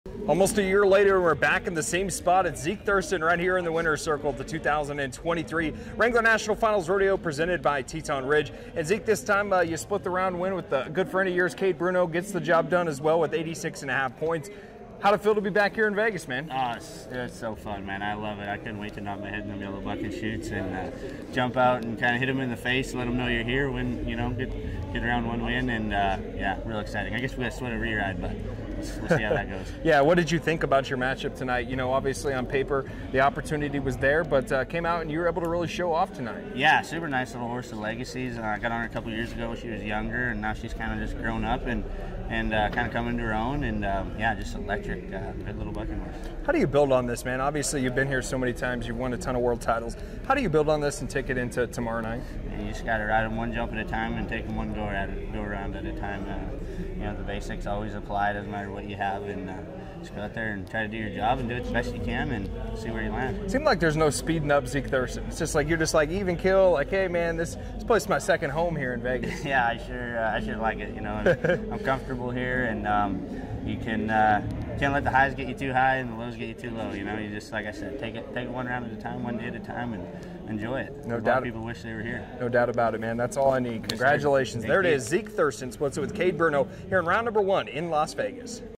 Zeke Thurston 2023 NFR Round 1 Interview
Zeke Thurston interviews with ProRodeo Insider Tanner Barth about his round 1 win at the Wrangler National Finals Rodeo presented by Teton Ridge.